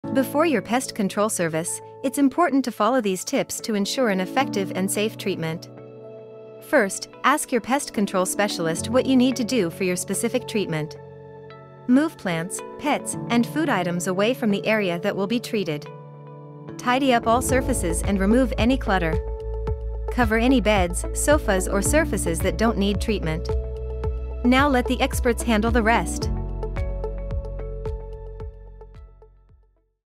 Prep Your Home For Pest Control Services
Pest control treatments can be stressful, but proper preparation makes all the difference for you and your pest control specialist. Follow these tips to make your treatment process simple and stress-free.